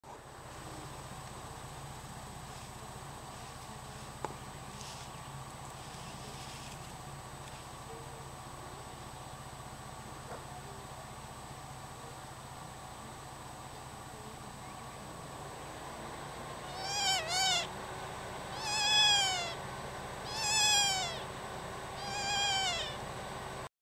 Ruffed Grouse
This is a female Ruffed Grouse (Bonasa umbellus) trying to ward me away from her chicks.  The call she makes is particularly interesting.  It sounds like a hurt animal.  Perhaps trying to draw me to her?  After the video she attacked!